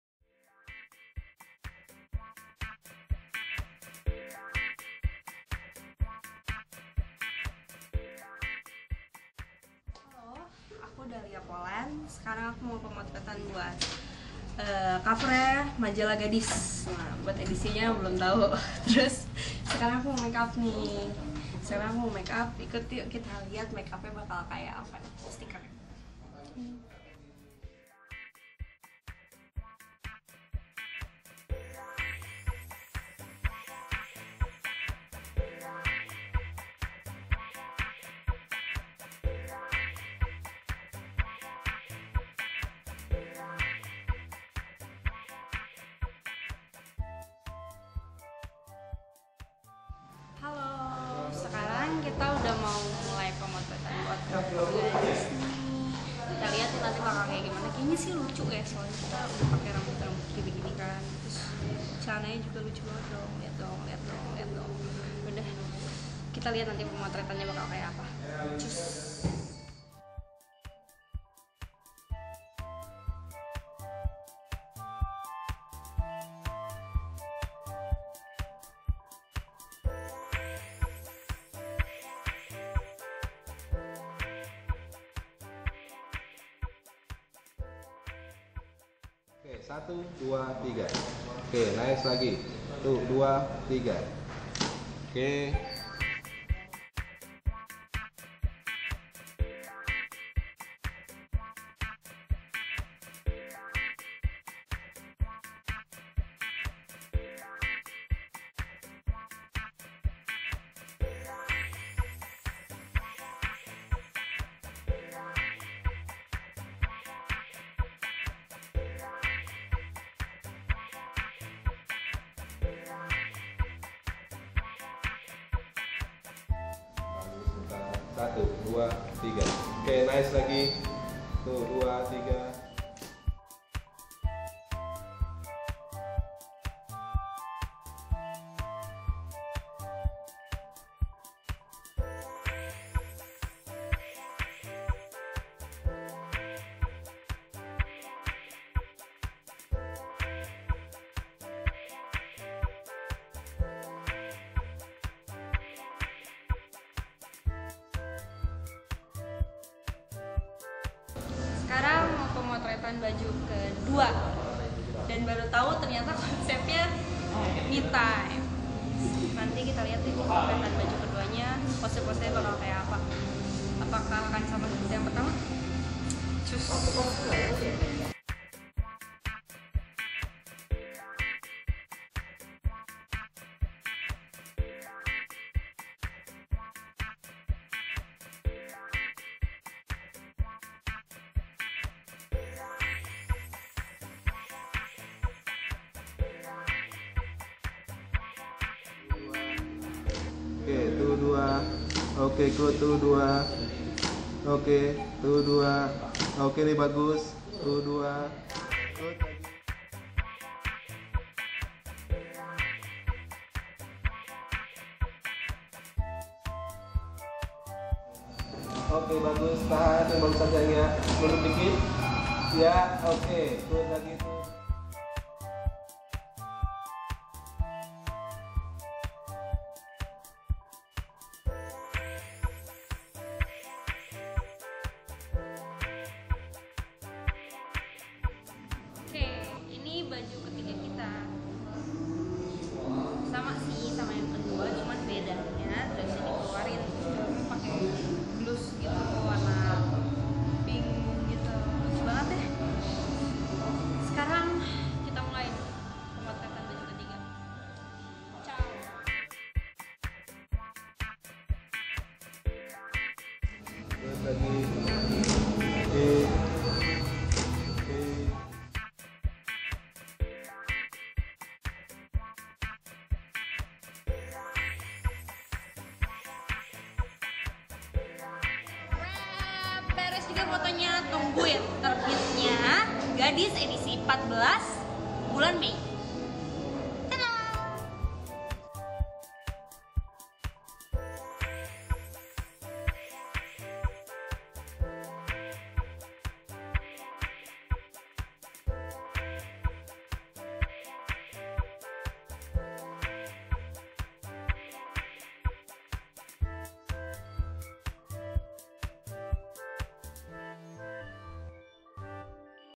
Behind The Scene - Dahlia Poland for Cover GADISmagz 14/2013
Ingat GADIS Sampul 2011, Dahlia Poland? Cewek yang sudah wara-wiri di dunia entertainment Indonesia ini bakal muncul lagi di sampul majalah GADIS edisi 14 yang edar 17 Mei 2013. Kita intip yuk, saat Dahlia melakukan pemotretan.